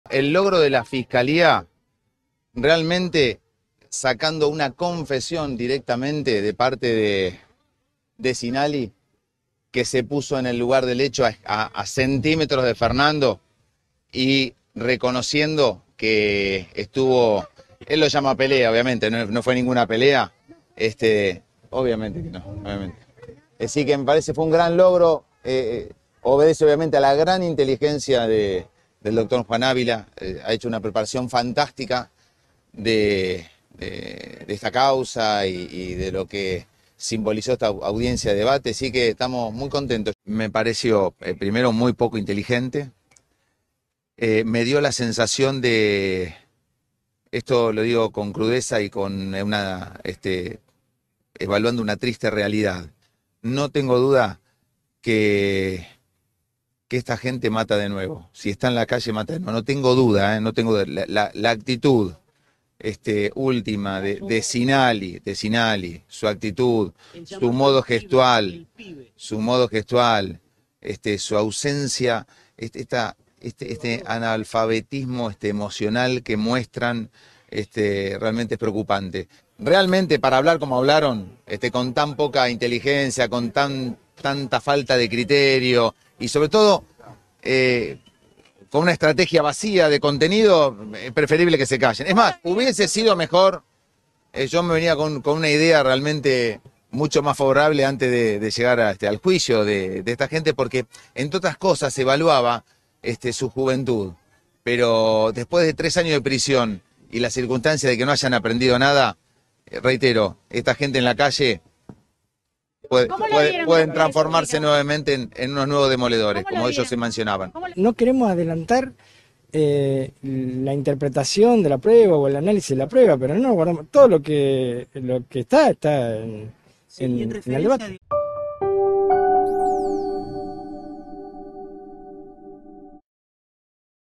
Para Burlando, la declaración de Blas Cinalli fue "una confesión"
Fernando Burlando, abogado de los padres de Fernando Báez Sosa, consideró hoy que la declaración de Blas Cinalli en la última audiencia del juicio fue "una confesión" ya que "se situó en la escena del crimen", y destacó el trabajo realizado por los fiscales y el tribunal a lo largo del debate.